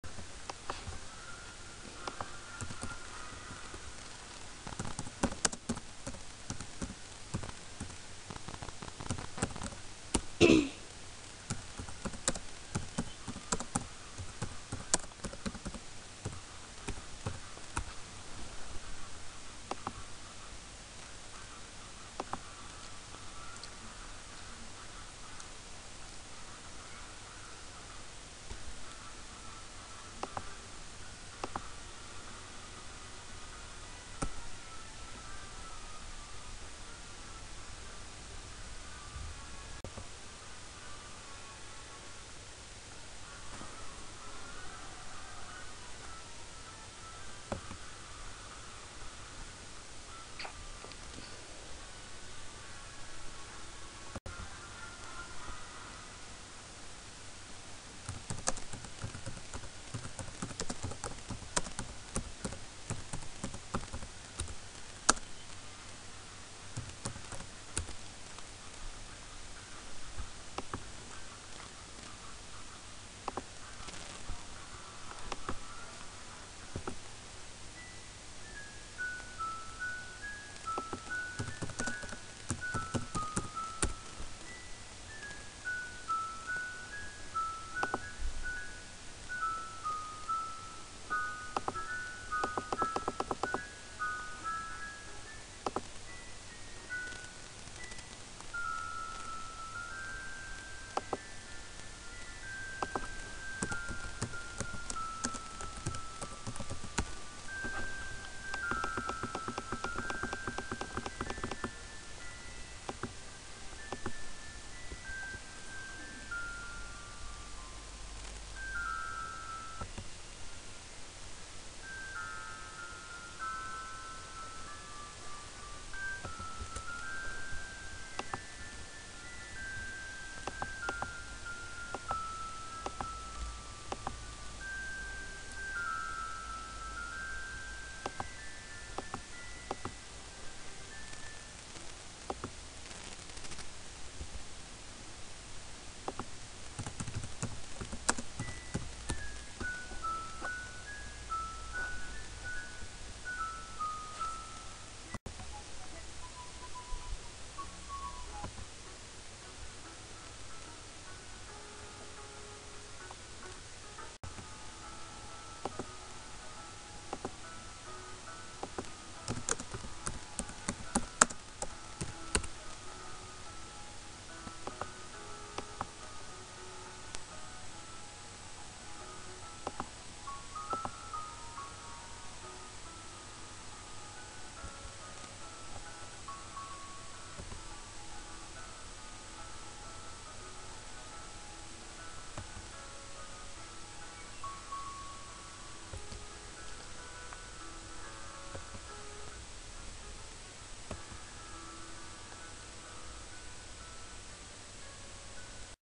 the meteorite orbs part 4 (FINAL PART)
sprattiehunt gets the last orb but the meteorite hits him